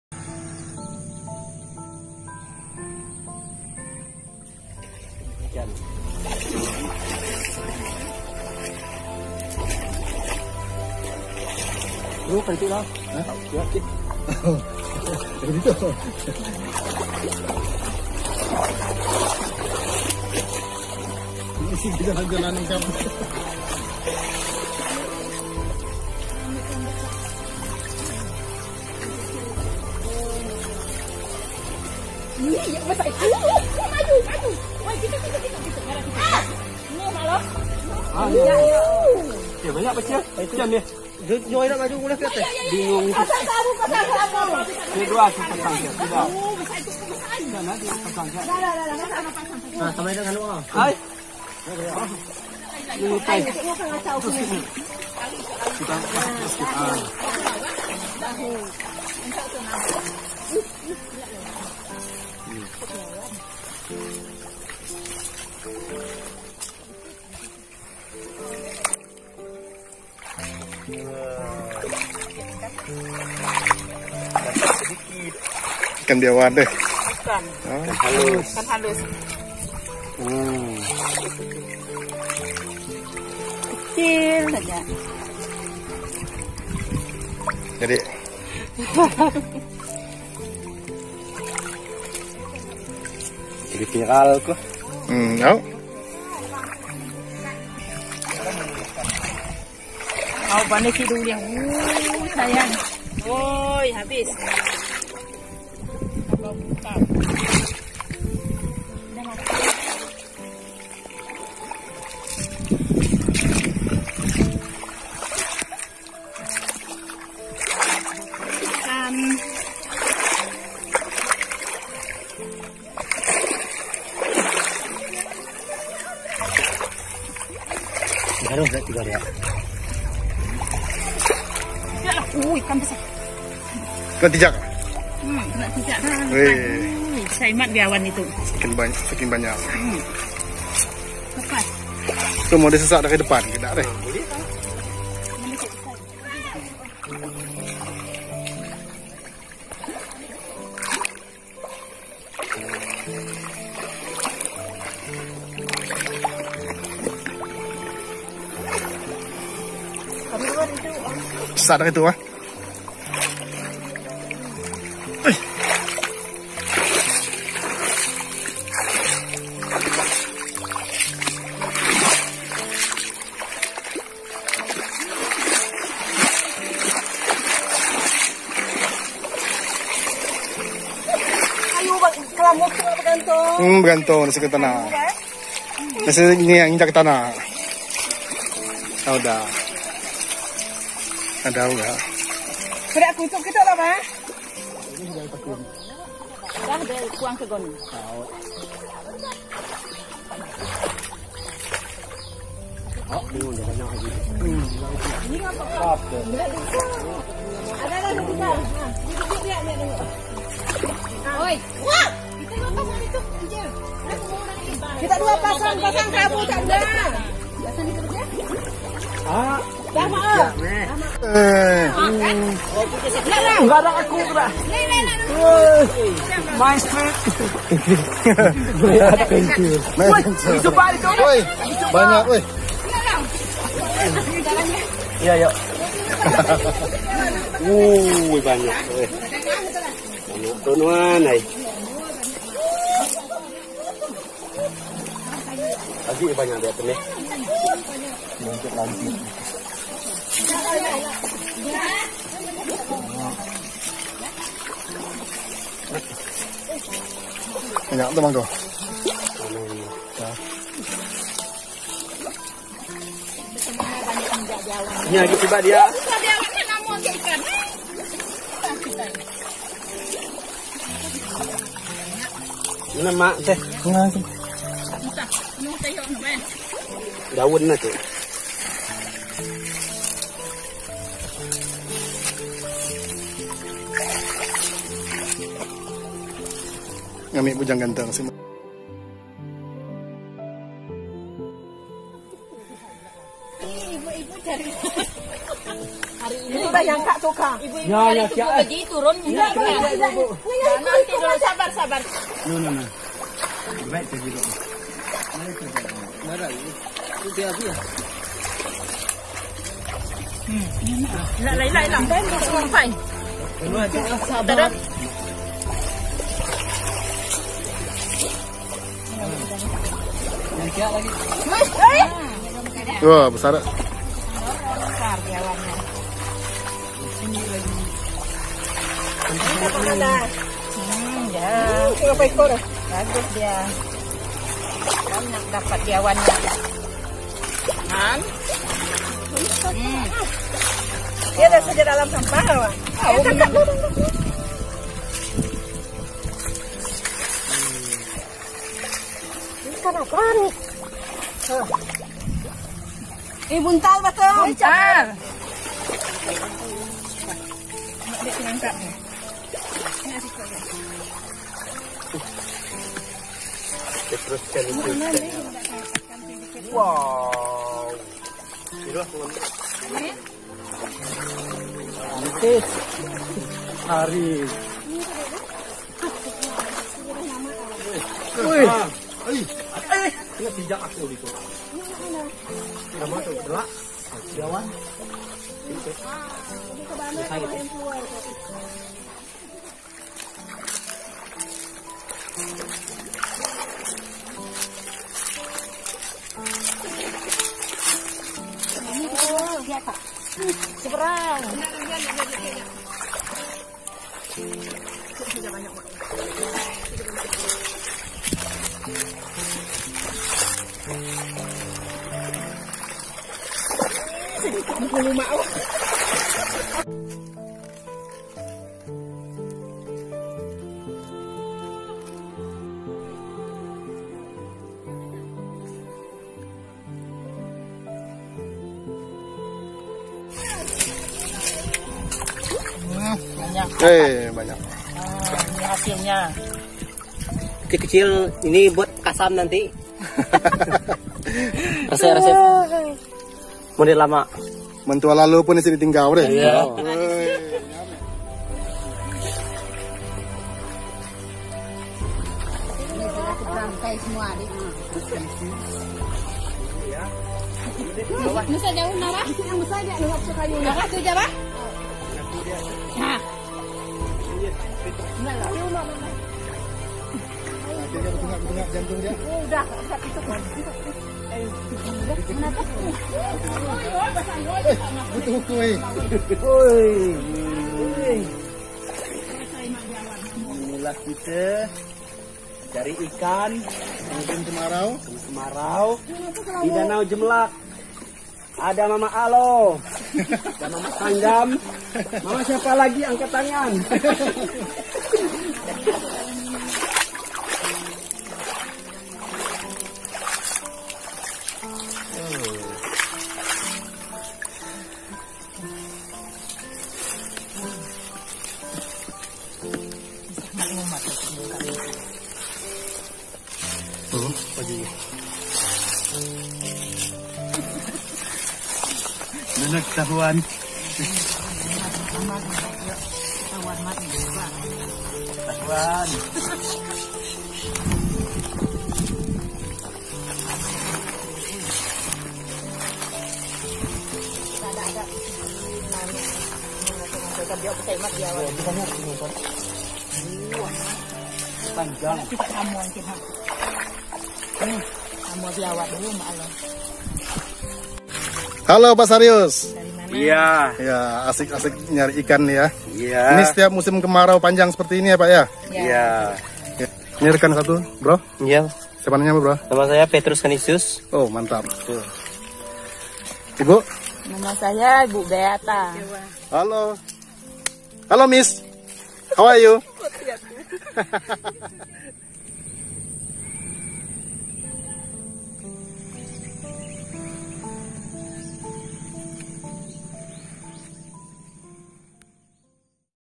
Trik_menangkap_ikan_yang_mudah_dan_cepat_mengunakan_kelambu_bekas_di_danau_jemelak
cara unik menangkap ikan